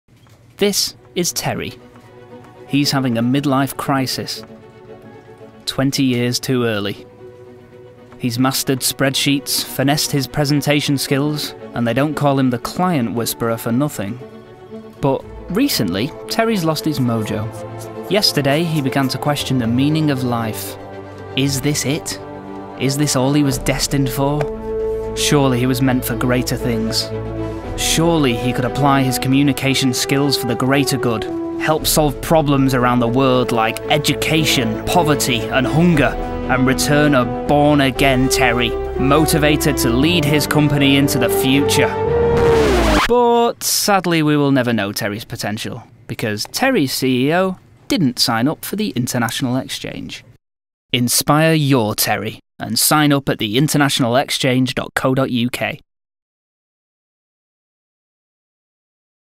Inspire Your Terry
The International Exchange (TIE) launches their first ever campaign, ‘Terry’.  The Terry Campaign message reminds CEOs that there are unlikely heroes hidden away in every company, blissfully unaware of what brilliant things they could achieve with their communication skills; they just need the opportunity to realize their true potential.

The campaign focuses on the power of the TIE program: to develop the future leaders of a company so that they can be exceptional, to demonstrate the power of the communications industry as a force for good, to provide people and companies with an opportunity to feel proud of their profession and the skills they have worked so hard to develop.

Film created by BBH London:
Creative Team: Laura Osbourne, Harry Orton & Robin Warman
Creative Director: Carl Broadhurst
Producer: James Southward, Jemima Bowers
Business Lead: Helen James
Account Director: Jennifer Omran